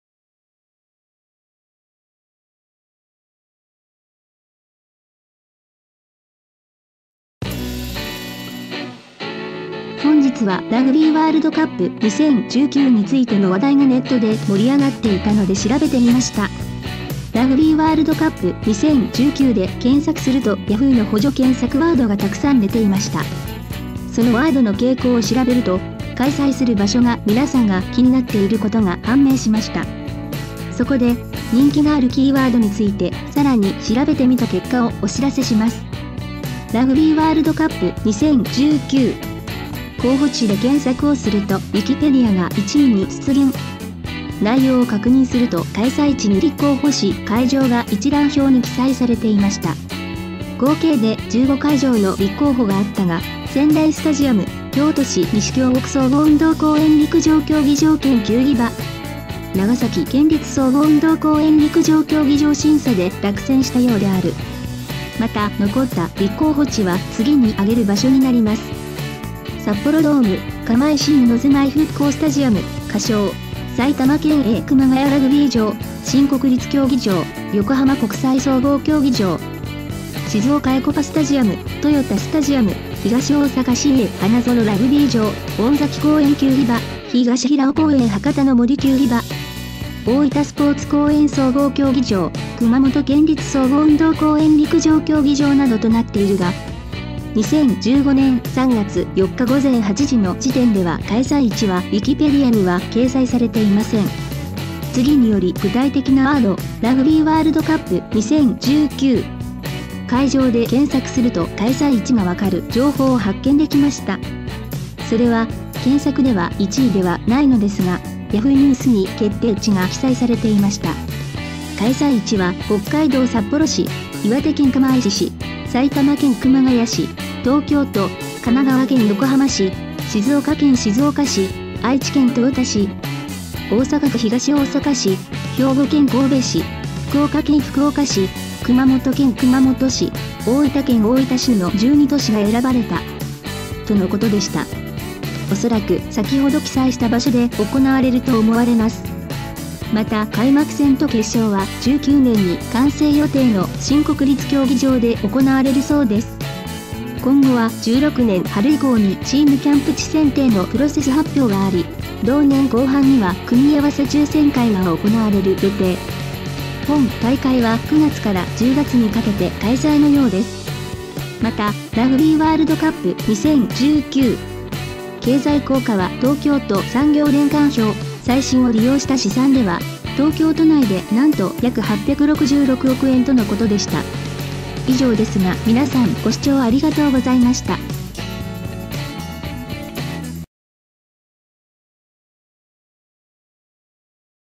ラグビーワールドカップ2019開催地が決定会場の候補地は大分・横浜など
ラグビーワールドカップ2019の開催地が決定したその候補地は大分、横浜、熊本、神戸など開催地に決定したと発表したが、その裏で落選した候補地もあった会場の情報やポスターは此方から・・・